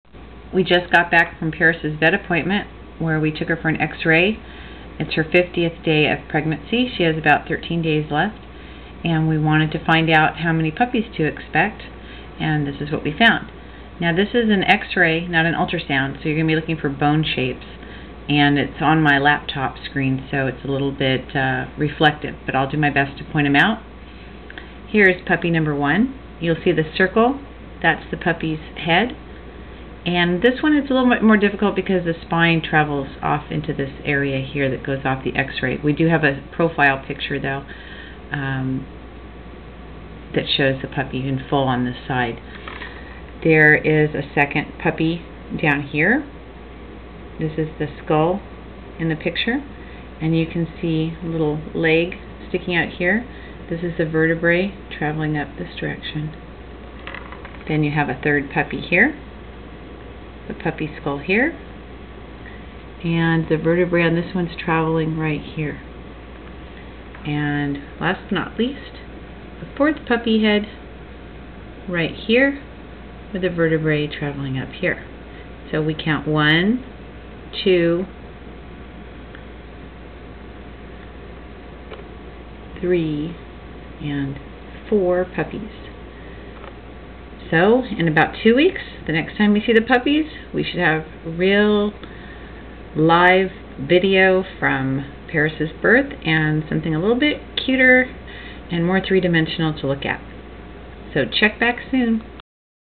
Video of our Yorkie, Paris', 50th day of pregnancy x-rays to determine how many pups she is expecting on or around New Year's Eve.